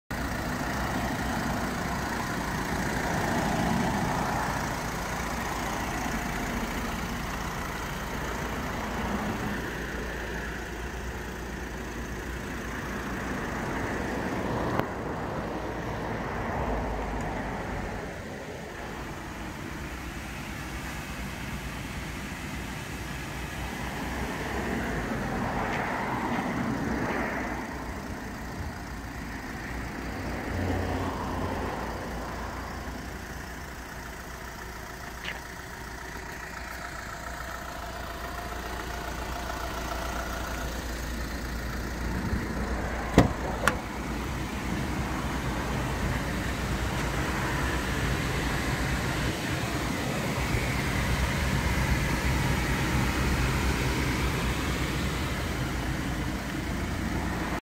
VW Transporter Panel Van 84hp SWB with Aircon and Tailgate finished metallic black.
Unique Transporters are delighted to offer VW Transporter Panel Van 84hp SWB with Aircon and Tailgate finished metallic black.
This van is offered in very good condition inside and out and drives superb
This Transporter comes with the following specification:
Euro 5
Tailgate
3 seat
Aircon
Very Low mileage 34k !
NO Vat
SWB
Unique Transporters Sportline Styling pack
New 20" Alloys and tyres
Upgraded Lowered suspension
Extendable Rac Warranty
Part exchange welcome
50+ Vw Transporters in stock
84bhp not enough? we can remap this van up to 170bhp prices from £339+vat